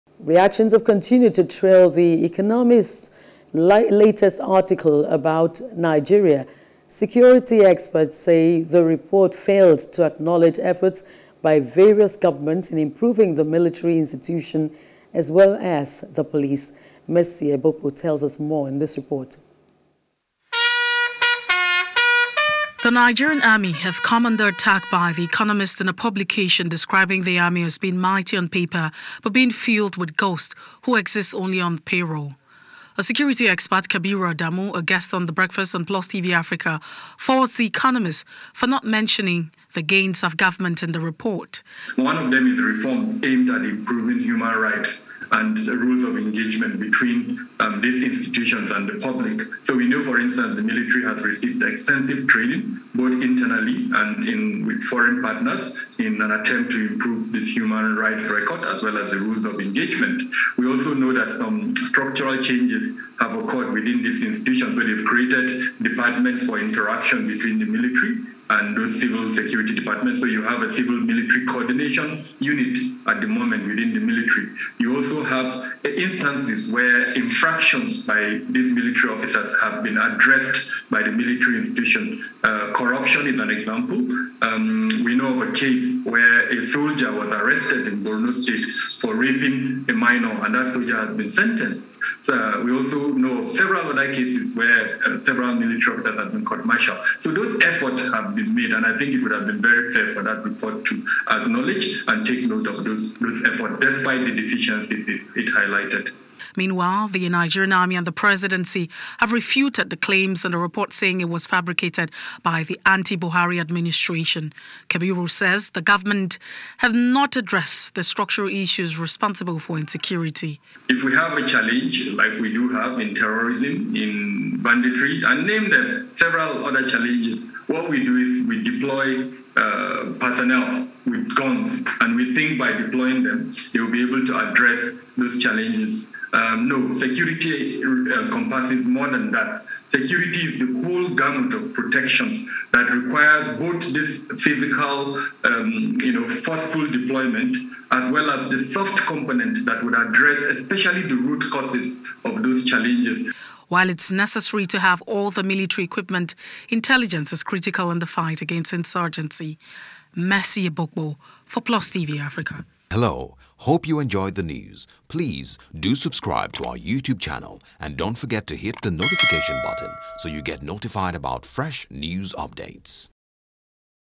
Security Experts Pick Holes In Published Article | NEWS
Reactions have continued to trail the Economist's latest article about Nigeria.